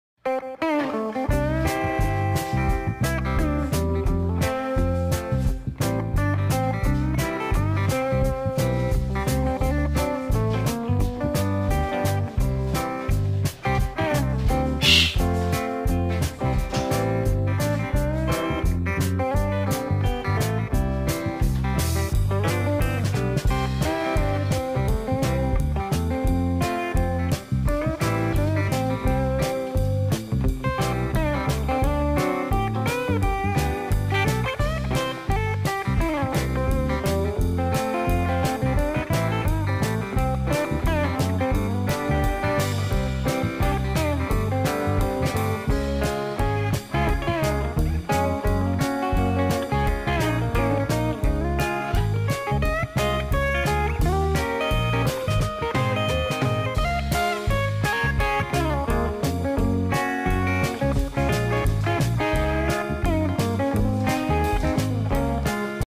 Silly macaw
My blue and gold macaw Templado just looooves hanging upside down :)